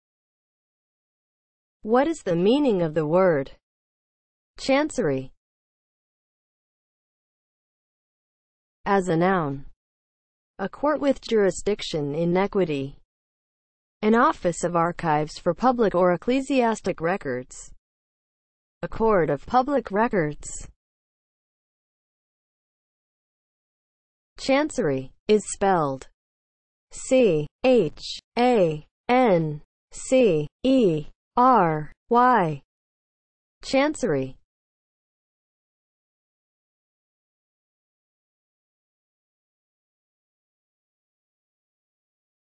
What is the meaning of the word CHANCERY?
Definition and spelling of the word CHANCERY.

(00:00) Intro
(00:07) As a noun
(00:22) Spelling

As a noun (00:07):
• a court with jurisdiction in equity
• an office of archives for public or ecclesiastic records; a court of public records